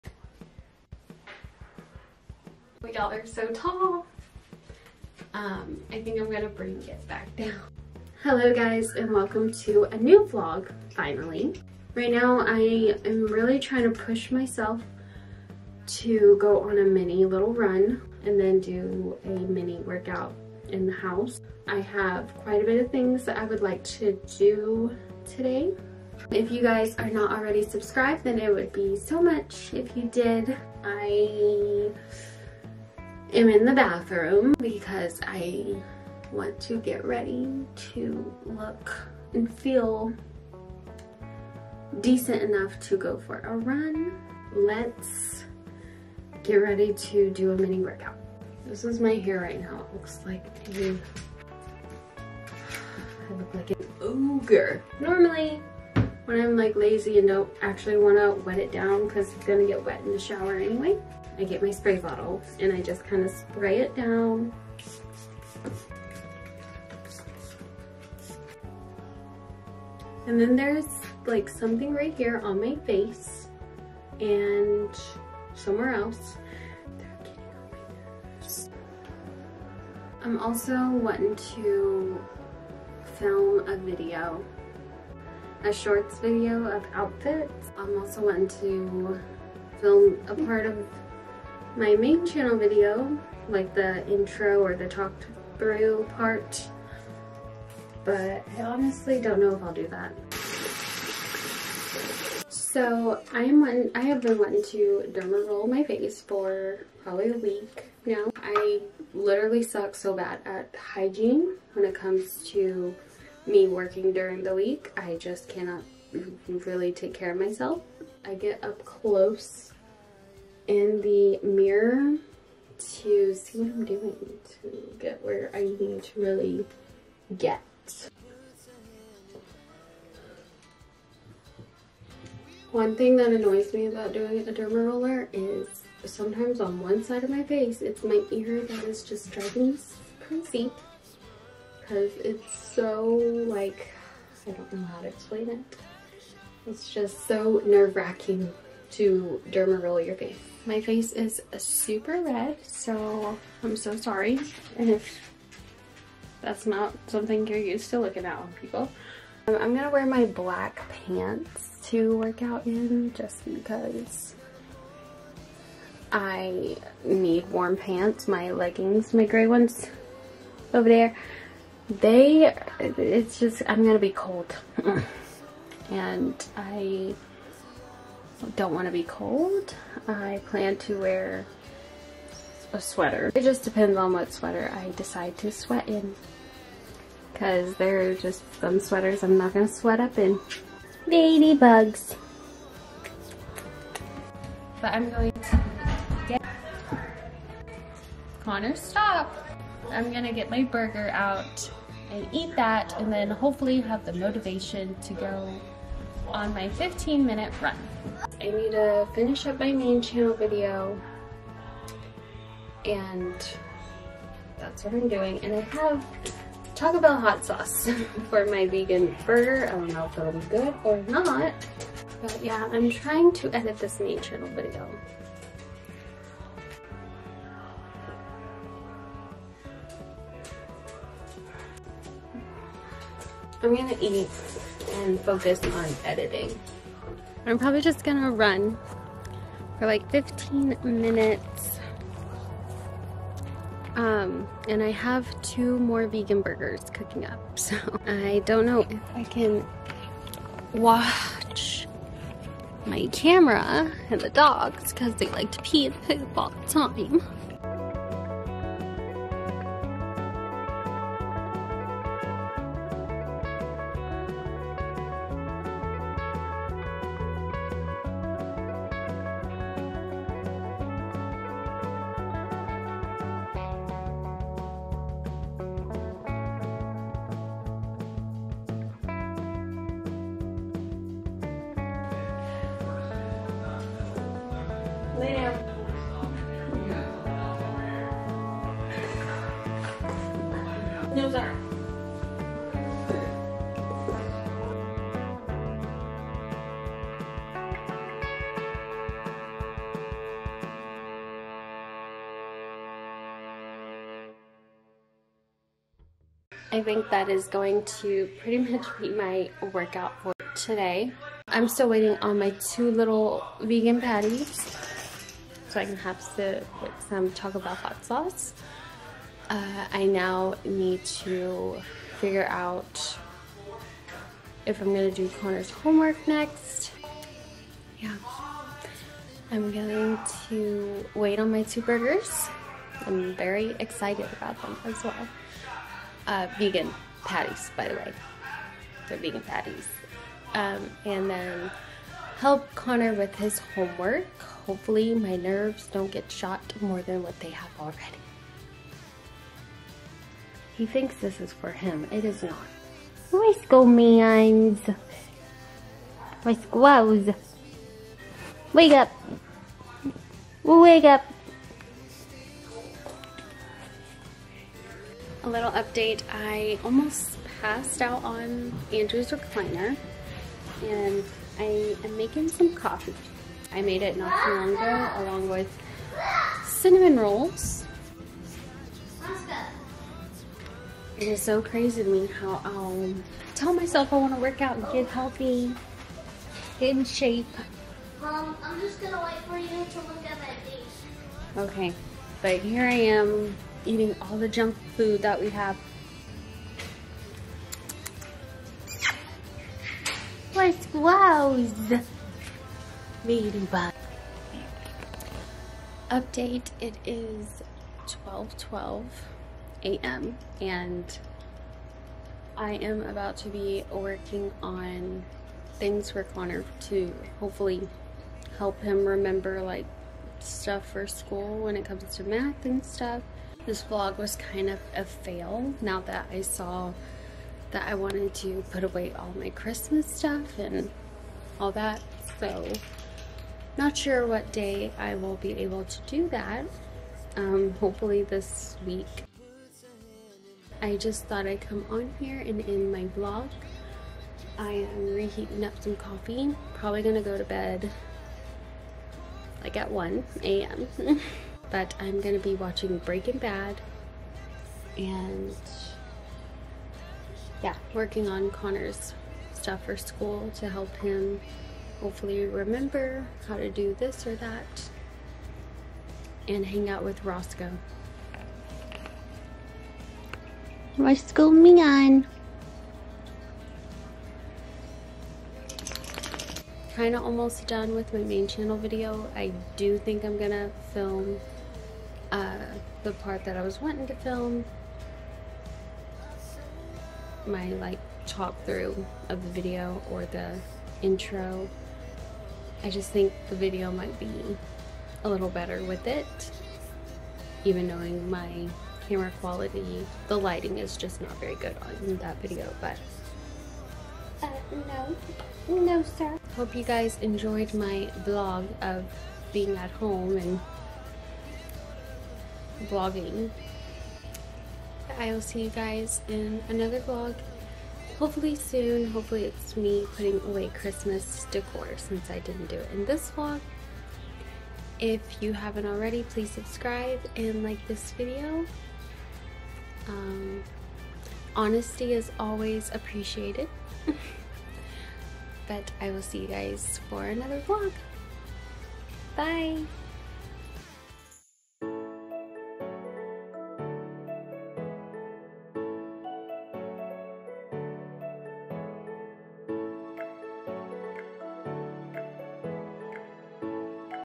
A Day At Home With Me
a day at home with me including my vegan plant based meat alternatives, derma rolling, workouts, etc.

Currently trying to figure it out as scheduling my videos and trying to film isn't always on key.

Canon EOS M50 Mirrorless Camera
Pixel MC-50 Microphone

\/Other things I've been loving/Seen in Video\/

3D Night Light for Kids

Guess 1981 Eau De Toilette Perfume

DERMA-E Advanced Peptides and Collagen Moisturizer

(purchased awhile ago but absolute fav)